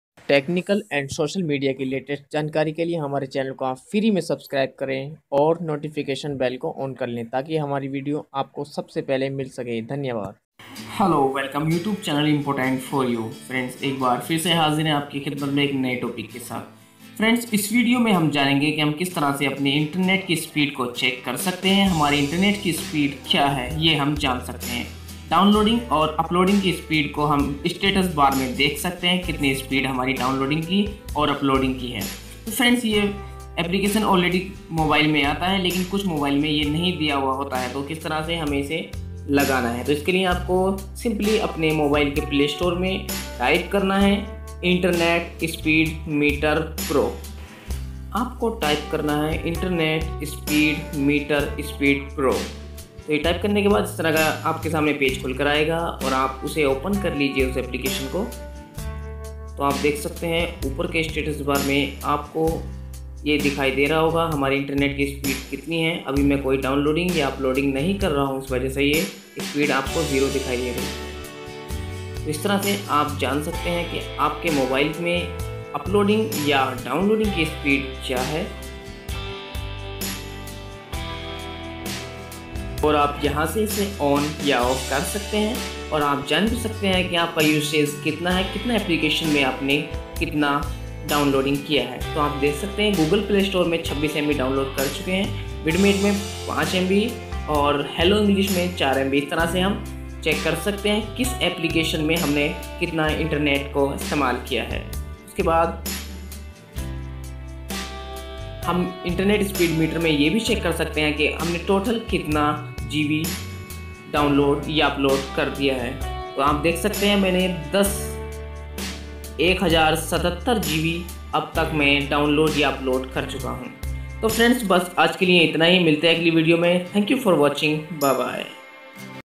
how to show internet speed on status bar all smartphone,internet speed test,NO ROOT.
i will show you/how to check internet speed on mobile.Show Internet Speed on Status Bar,
(1)enable internet speed,Internet speed in statusbar,
(2)Show Internet Speed,internet speed,No Root,Show Internet